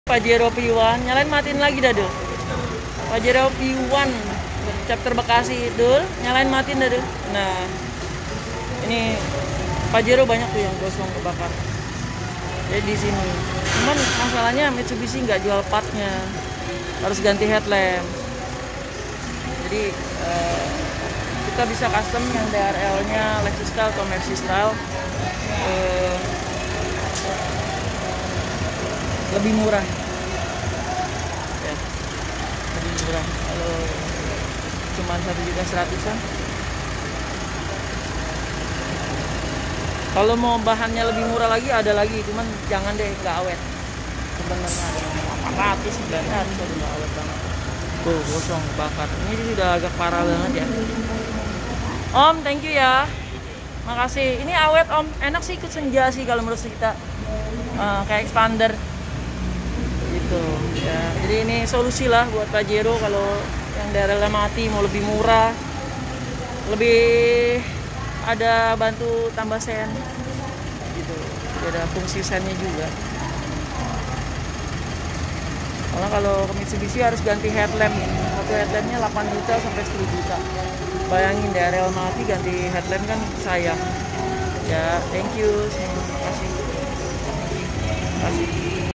Mitsubishi pajero sport dakar problem lampu drl redup kuning mati solusi terbaik harga terjangkau
Mitsubishi all new pajero sport :
Problem oem drlnya redup kuning gosong mati, solusinya replacement with drl Lexus or Mercy style.

Drl putih + sein flash mercy / sein sequential lexus + Devil eyes (putih, biru, ice blue, merah, hijau, ungu)

Rp1,2-1,5jta termasuk biaya installasi.